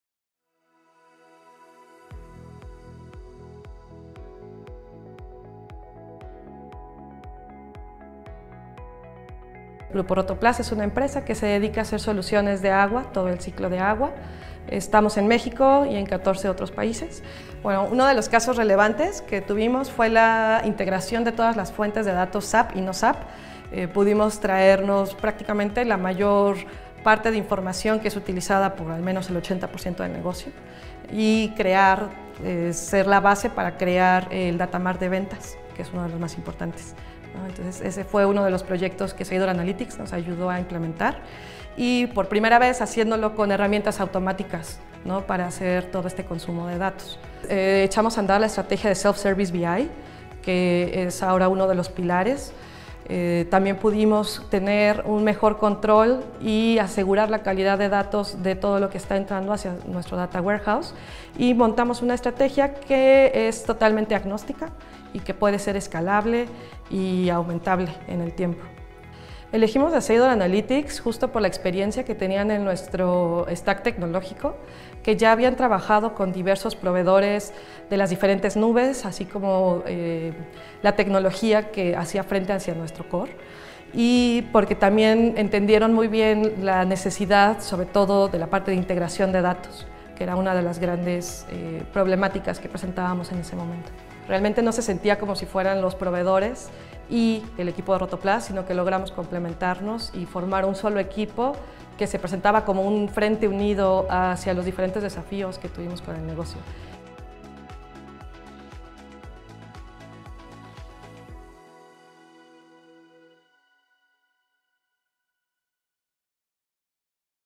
Conoce el Caso de éxito de Rotoplas y su Estrategia en Data&Analytics
Grupo Rotoplas es una empresa multinacional mexicana dedicada a la fabricación de tanques de almacenamiento y filtración de agua fundada. Confiaron en nuestro equipo para la implementación de Analytics. Enterate de qué forma lograron cambiar su estrategia de datos y cuales son los beneficios que obtuvieron.
Transcripción